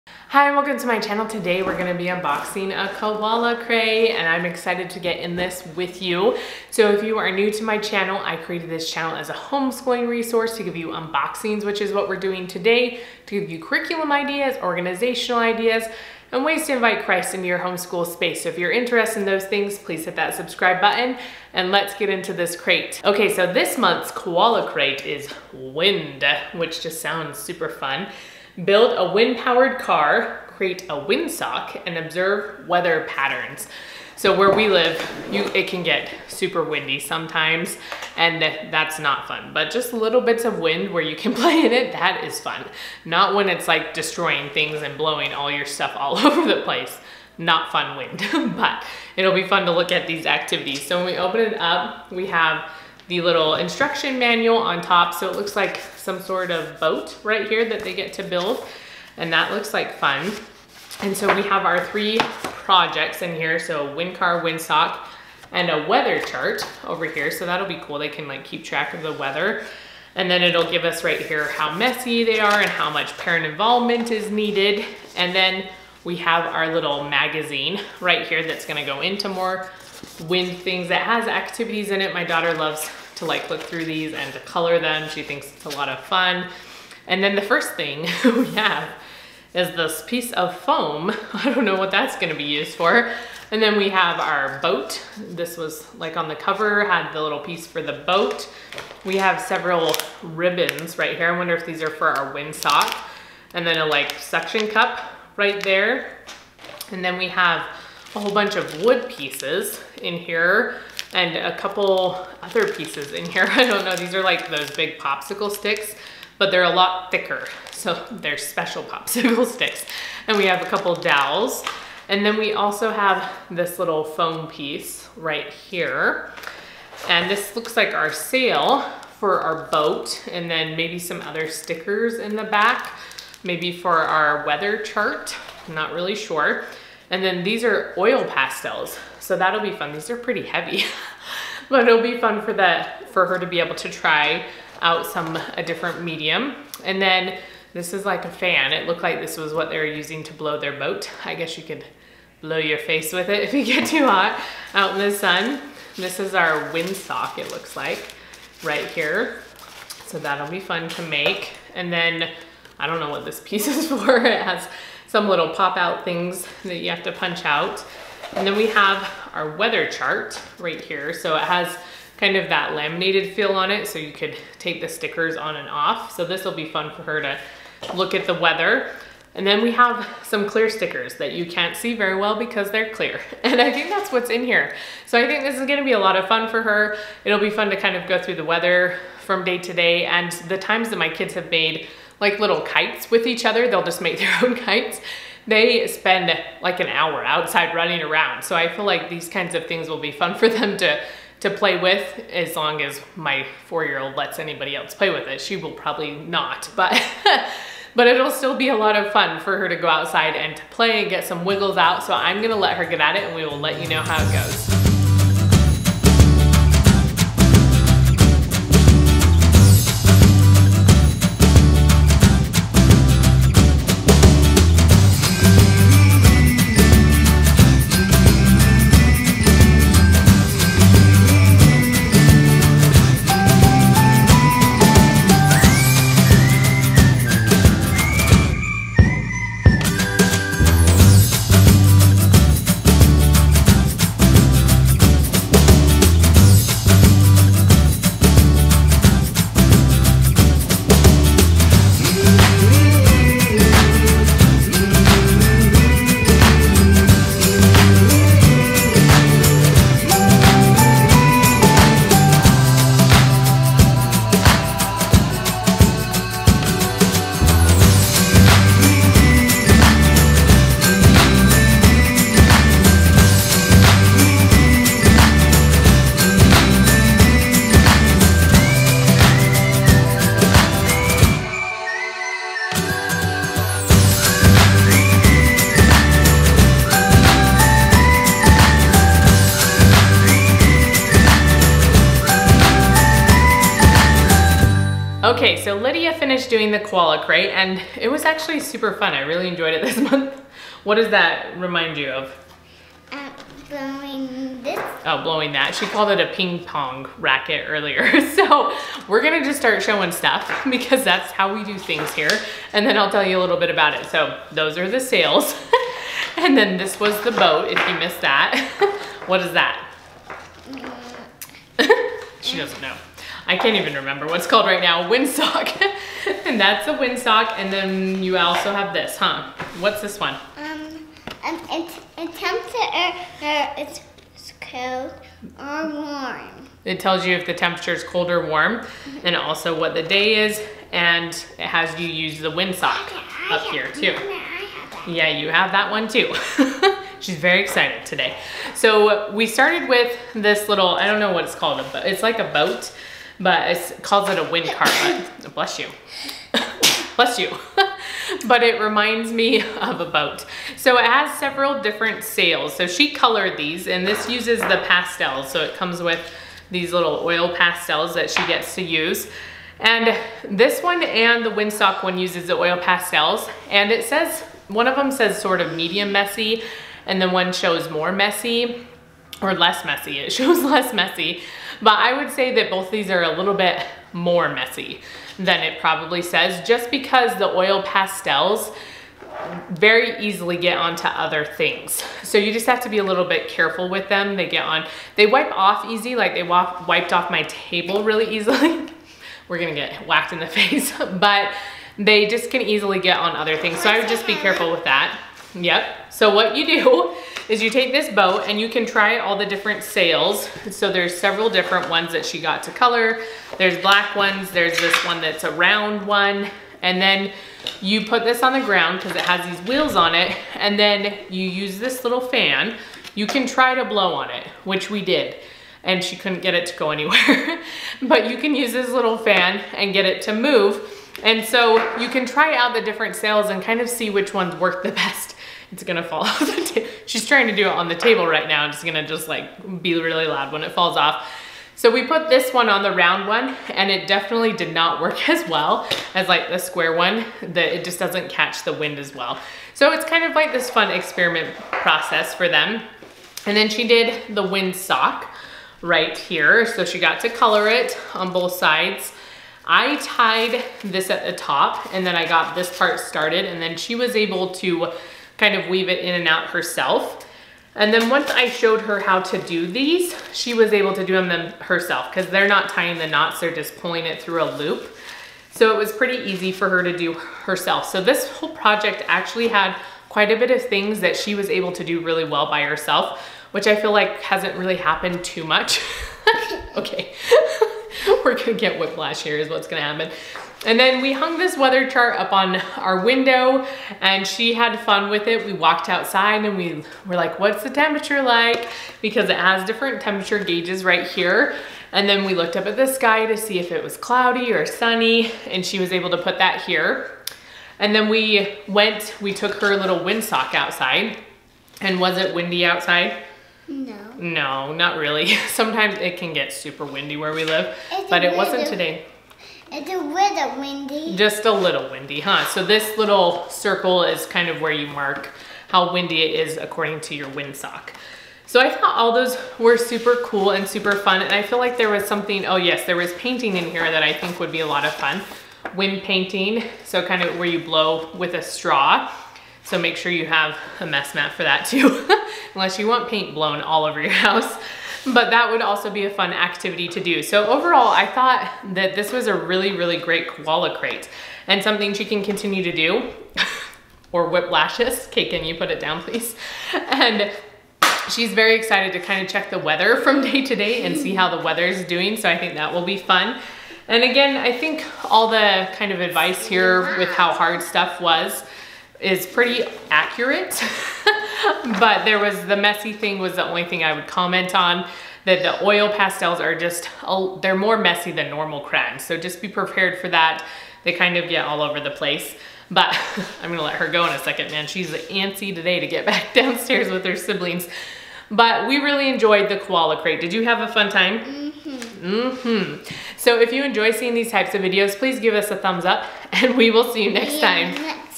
Koala Crate Unboxing May 2021 - Wind
Hi friends, today I am sharing an unboxing of the May 2021 Koala Crate from Kiwico. This is a monthly subscription box for 2-4 year olds and my 4 year old loves it. These boxes encourage kids to be creative, practice fine motor skills and provide a little break for a busy homeschooling mom.